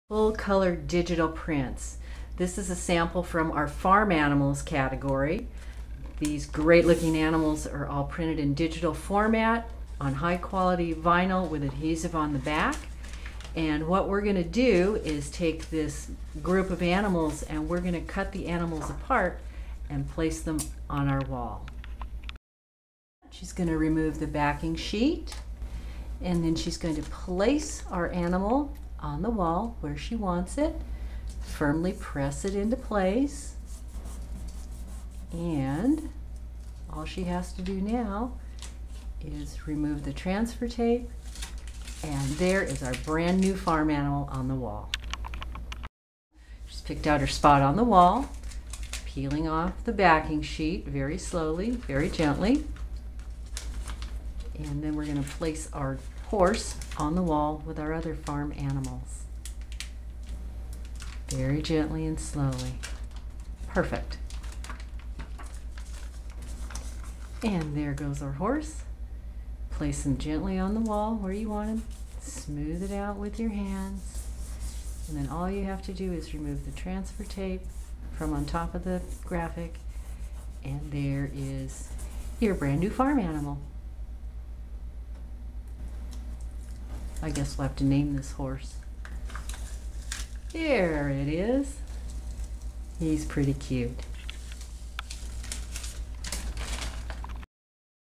Installing Digital Wall Graphics - Wallwords.com
Sabrina from Wall Words shows you the correct way to install your digital graphic in you home or office.